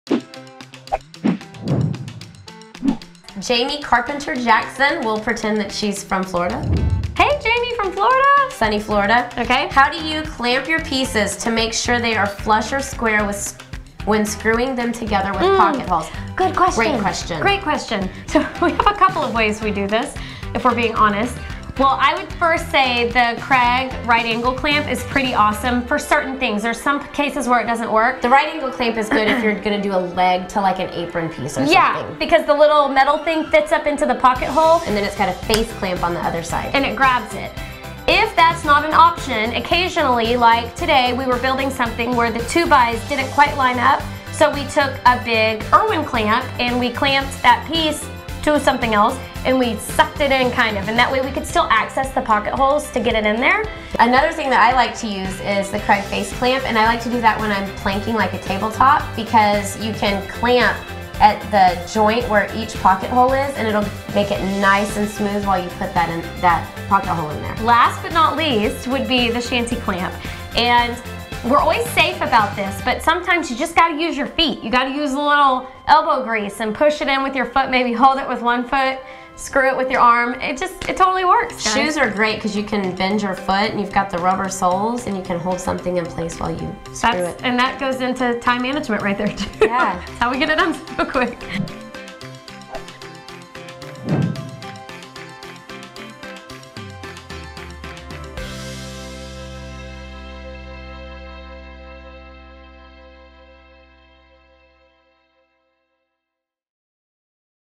How to clamp boards when using a Kreg Jig Q&A
Today we are sitting down with with coffee (of course) and answering questions about how we clamp boards when we use our Kreg Jigs.  We get questions about this ALL the time, and we hope this helps you out just a little bit!

Here are links to the three clamps we suggested.  You can find lots of tutorials on our site that use each of them!

Kreg Right Angle Clamp

Kreg Face Clamp

Kreg AutoMaxx Face Clamp

Irwin Bar Clamp